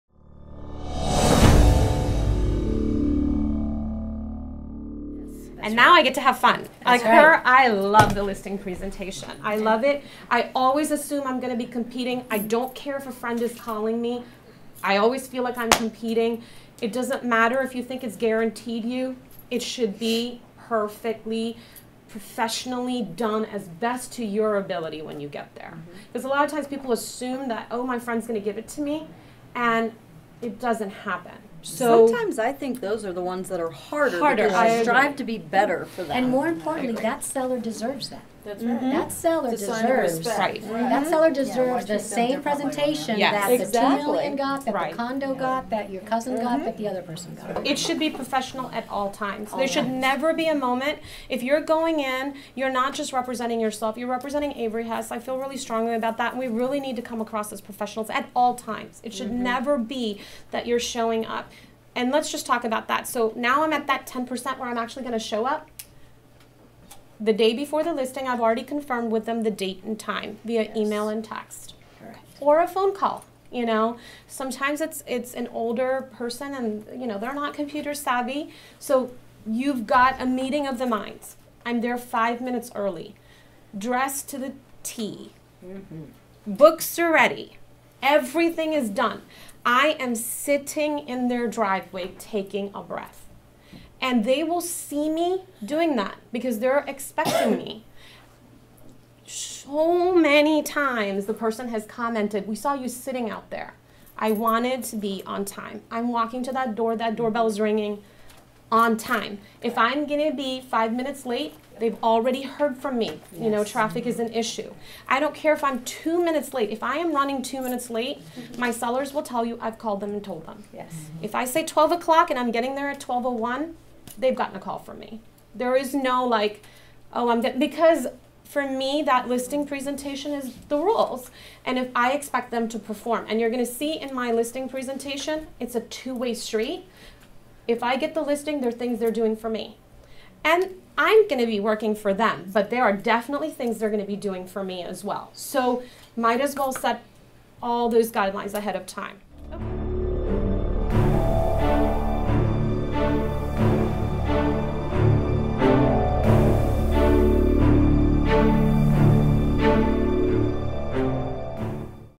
Before You Step in the Door - Part 18/24 - Master the Listing Presentation
Once the presentation day arrives the your execution is critical to your success. Seema Rodriguez let's us in on how she makes sure she's up for the challenege before she even steps in the door.